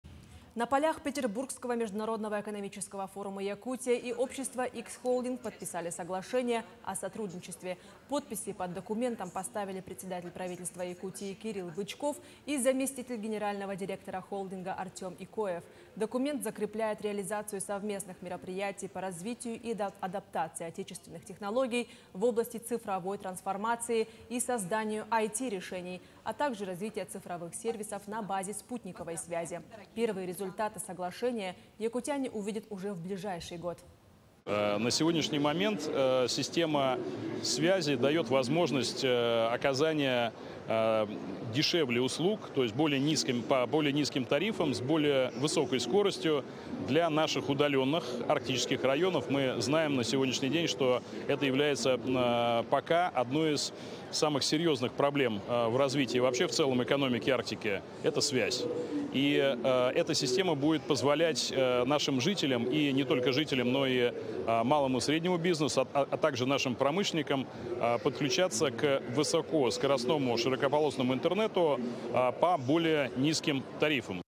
Якутия и ООО «Икс-Холдинг» подписали соглашение о сотрудничестве на полях ПМЭФ-2024
Подписи под документом поставили председатель правительства Якутии Кирилл Бычков и заместитель генерального директора холдинга Артем Икоев. Документ закрепляет реализацию совместных мероприятий по развитию и адаптации отечественных технологий в области цифровой трансформации и созданию востребованных отраслевых IT решений, а также в сфере развития цифровых сервисов на базе спутниковой связи низкоорбитальной группировки.